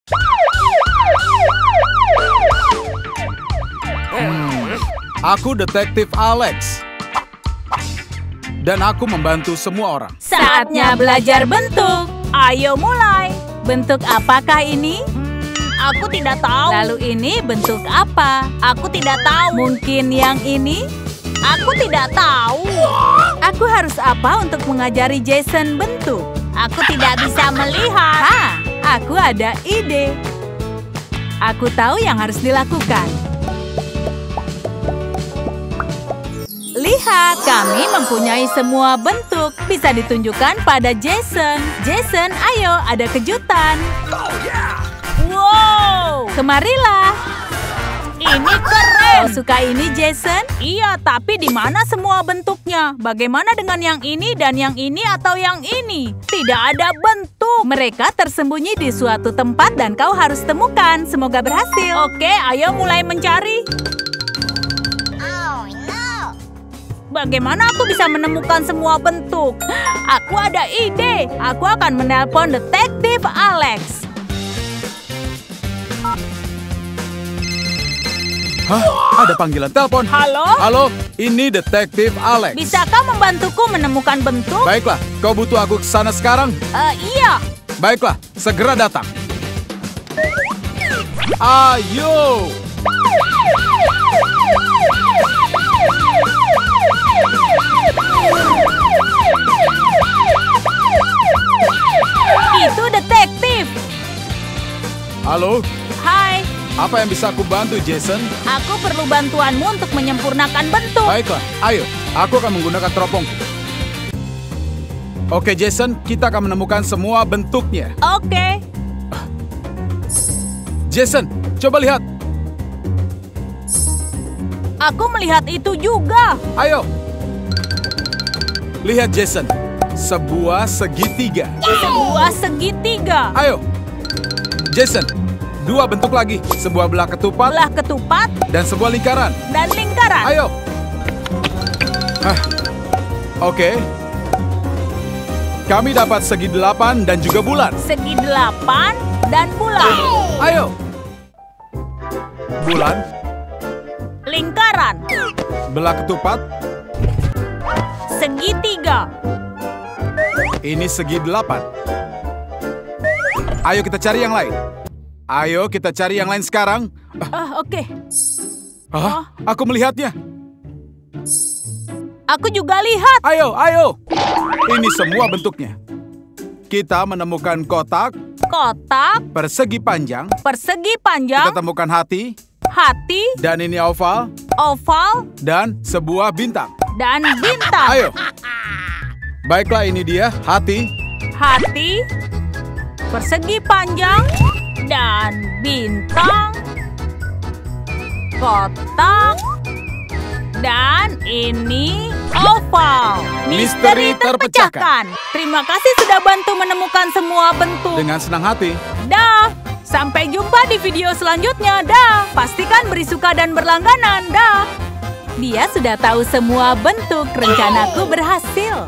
Jason dan Detektif Alex belajar bentuk sambil bermain! Video pendidikan untuk anak-anak
Jason ingin mempelajari bentuk-bentuknya tetapi tidak dapat mengingatnya. Dia memanggil Detektif Alex untuk meminta bantuan. Dalam bentuk permainan, mereka mempelajari bentuk-bentuk tersebut dan Jason dengan mudah menguasai segalanya. Video pendidikan untuk anak-anak